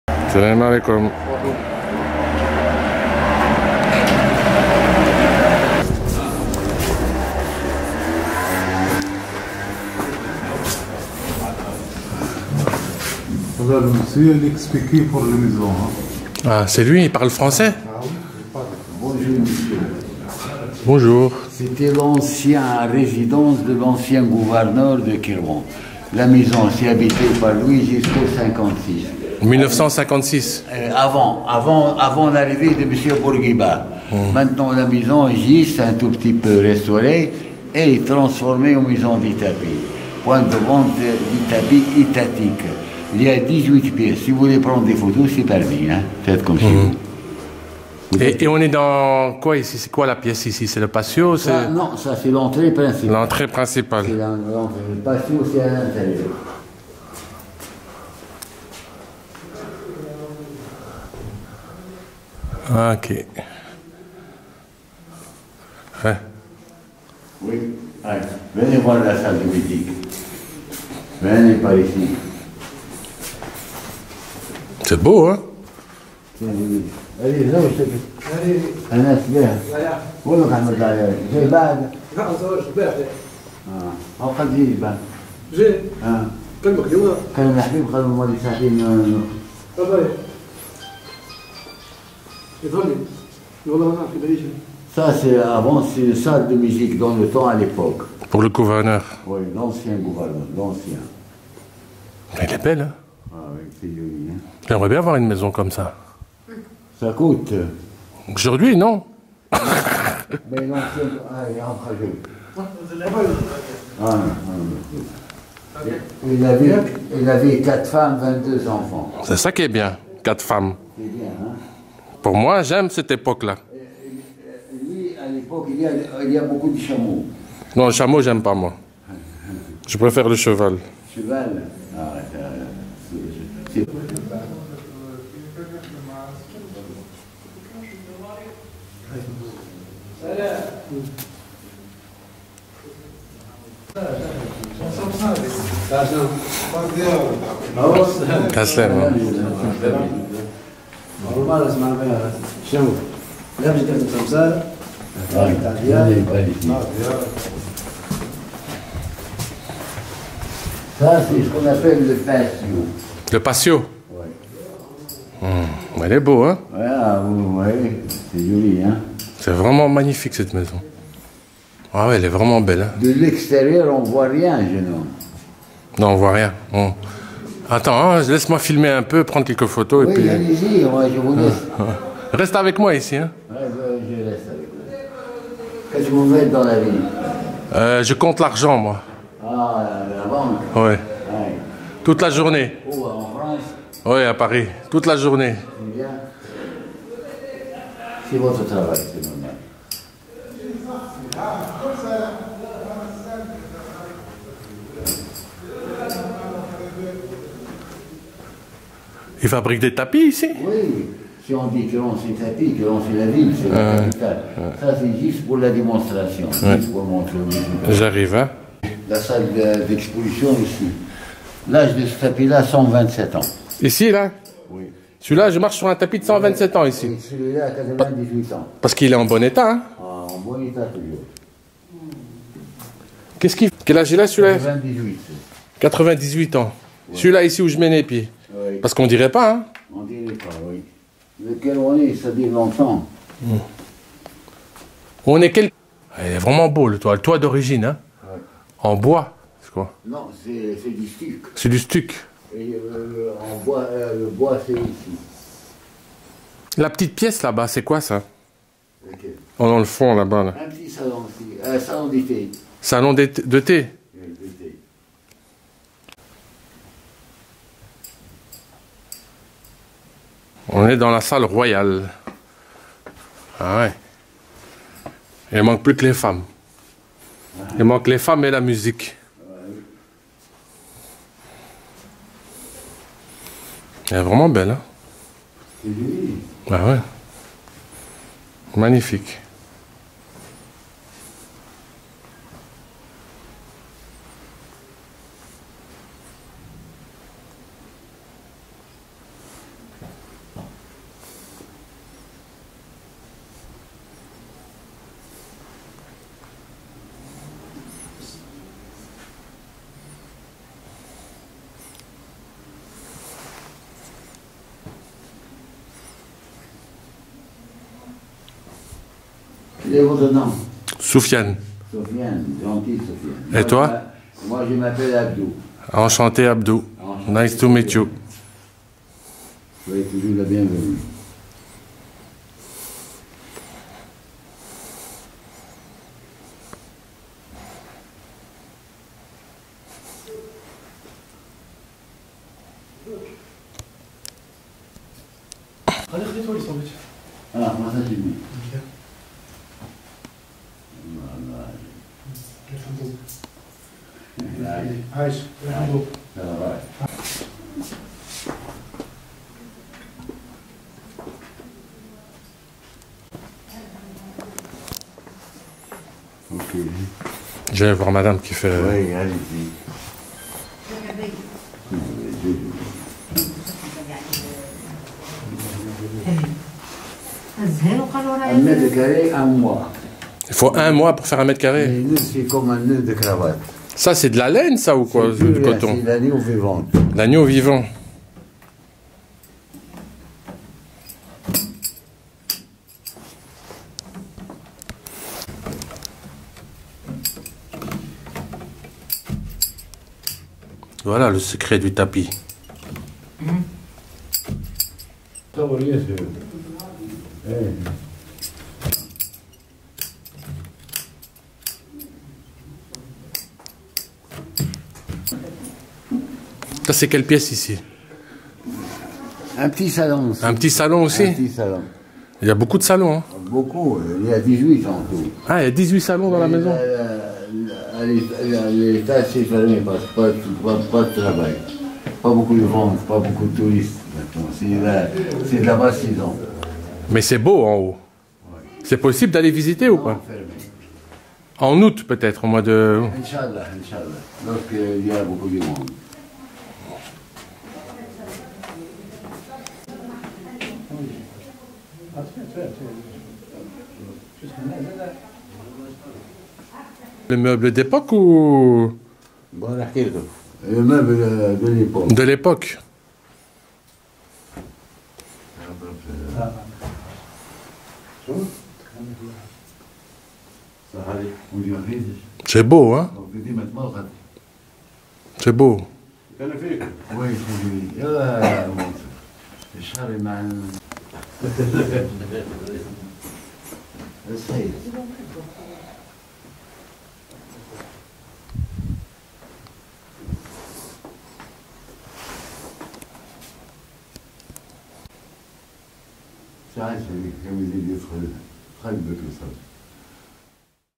Visite de la maison du Bey ou du gouverneur de Kairouan
C'est la plus belle maison de Tunisie que j'ai vue jusque ce jour, ont en trouve pas ailleurs. Elle est d'une beauté et d'une exceptionnelle architecture. Elle a su garder son charme discret à l'intérieur des murs. Si vous êtes de passage dans la médina de Kairouan, surtout ne manquer pas de visité la maison, elle vaut le détour.